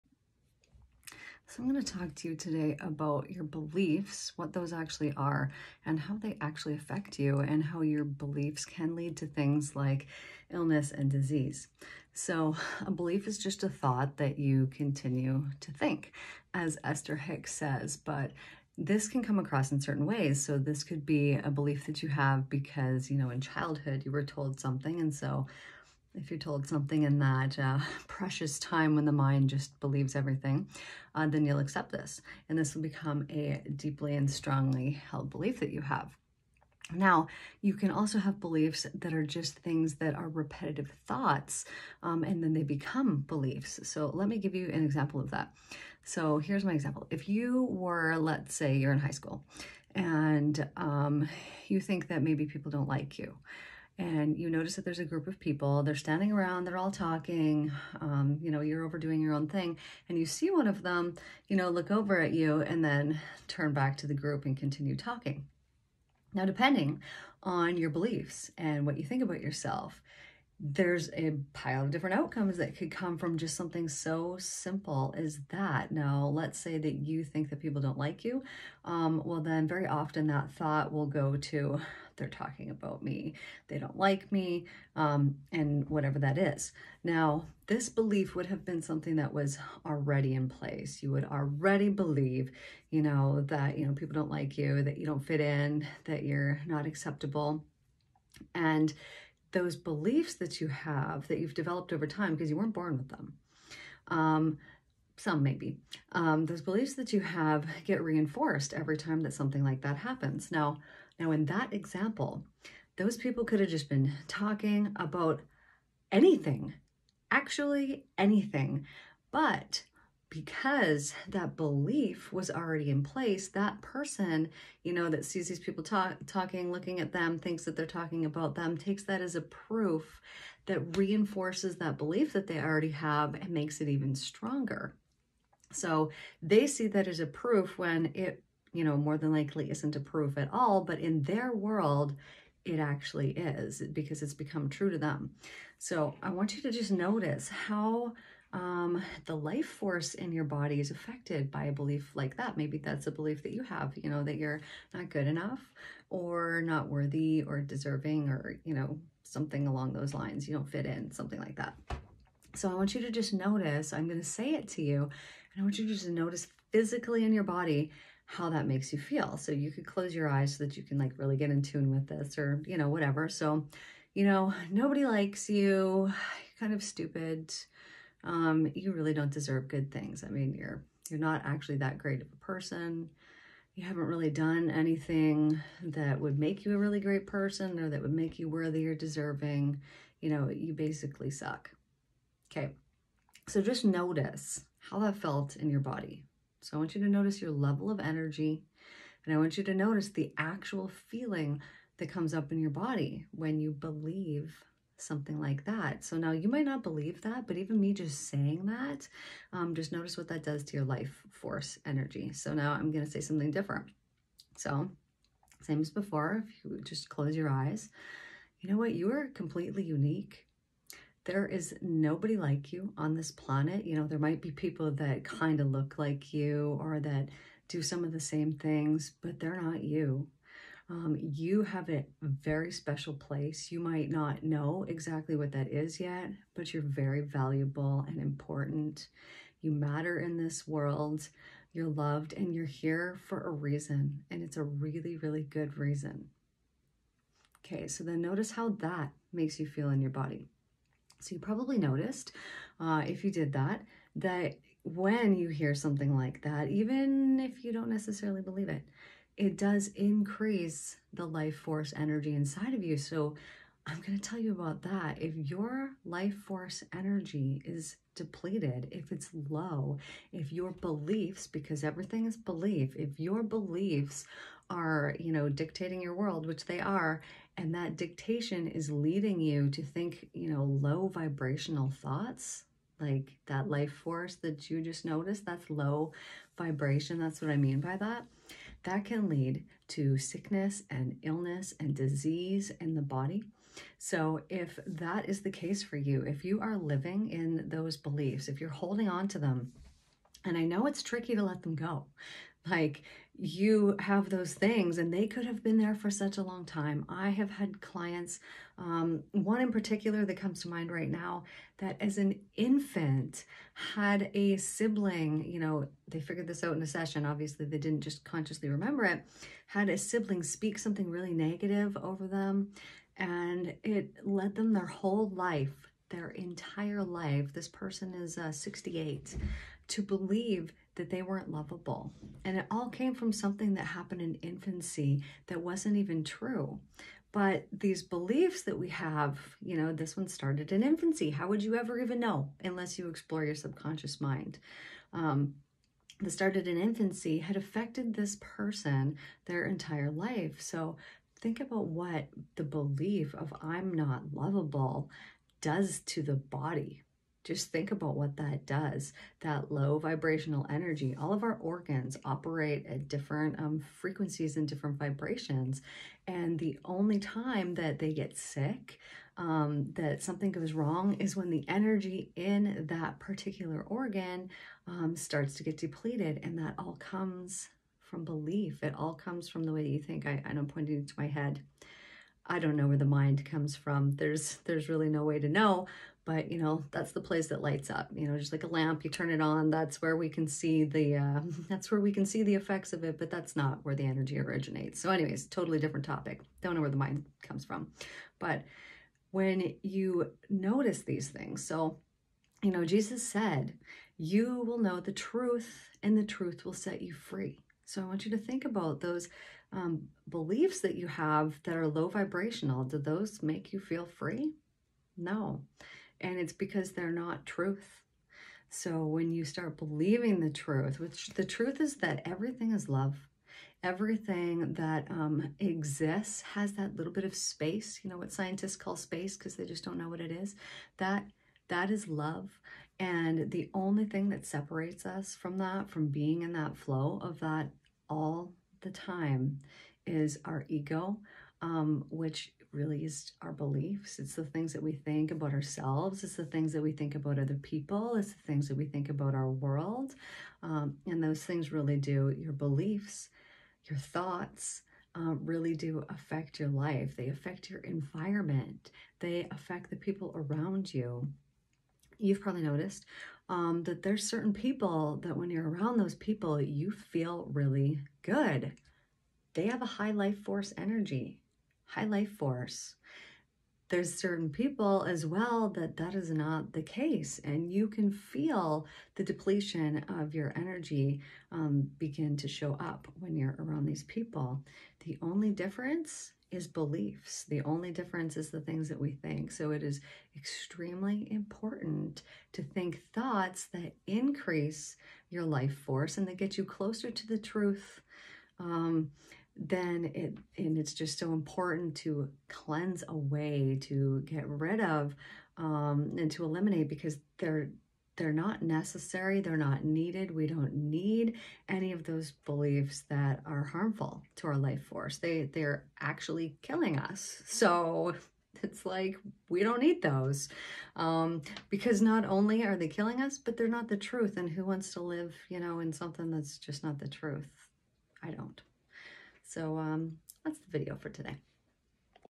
How beliefs affect your life force energy
Your beliefs can make you vibrant and healthy or make you sick eventually leading to disease.